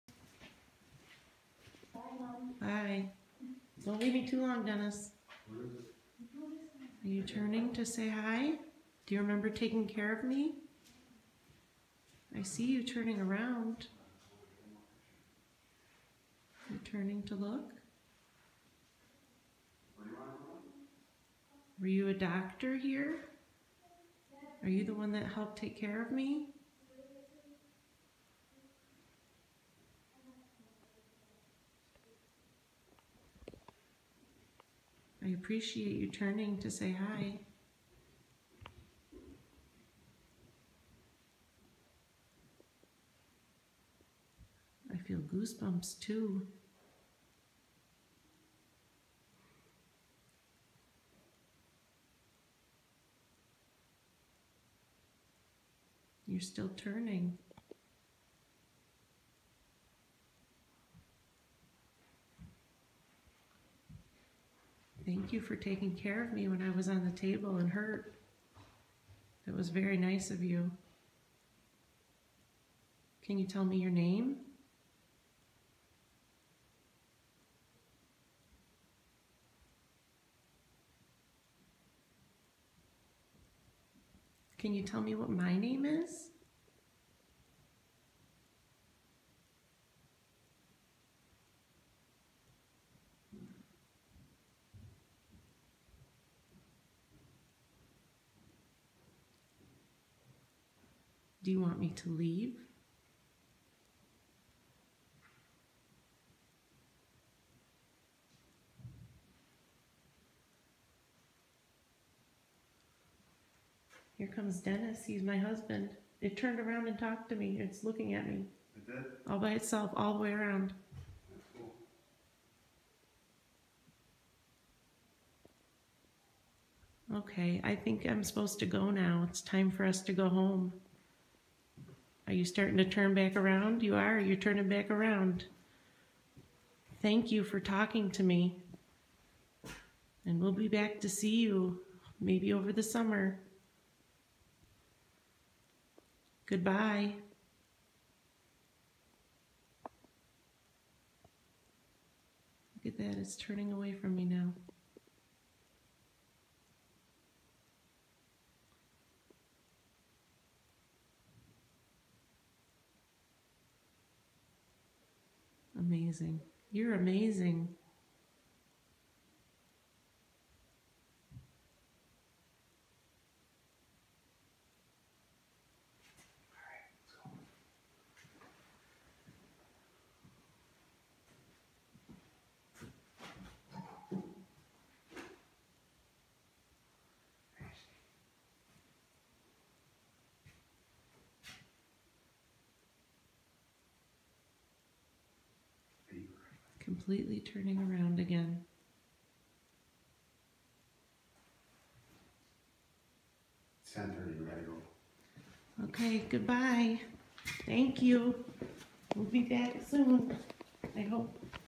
Rolling Hills Asylum, morgue
When everyone else left the morgue, this suit turned around to face me. When my husband returned, the suit turned back around again. I think it liked that I was alone.